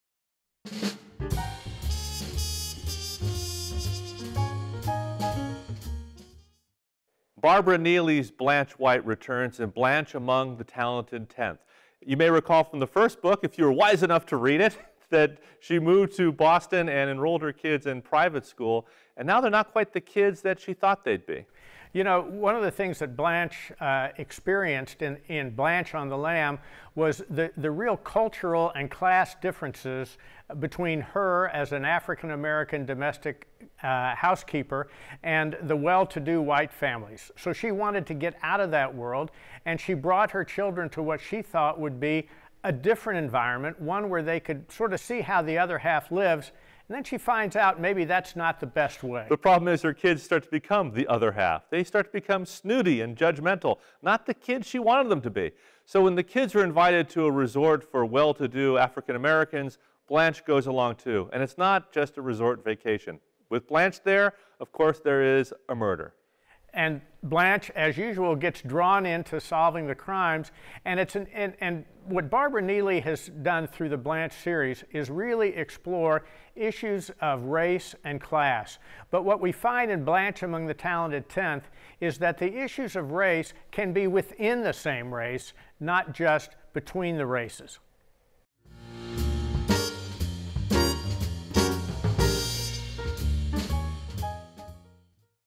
Brash Books: BLANCHE AMONG THE TALENTED TENTH by Barbara Neely
Lee Goldberg & Joel Goldman introduces Barbara Neely's BLANCHE AMONG THE TALENTED TENTH.

THE SECOND, GROUND-BREAKING MYSTERY FEATURING AFRICAN-AMERICAN MAID AND AMATEUR SLEUTH BLANCHE WHITE BY AGATHA, ANTHONY, AND MACAVITY AWARD WINNING AUTHOR BARBARA NEELY

When Blanche White moved north to Boston, she believed it would be a better place to raise her kids, especially after she got them into an elite private school. But now her children are becoming snobby and judgmental, acquiring more attitude than education. So when she and her kids are invited to Amber Cove, an exclusive resort in Maine for wealthy blacks, Blanche jumps at the chance to see how the other half lives and to perhaps stop her kids from turning into people she doesn’t want to know. When one of the guests kills himself, and another is electrocuted in her bathtub, Blanche becomes an accidental detective once again, using her sharp wit and keen social insight to peel back some disturbing color and class distinctions within the black community that may have driven someone to murder.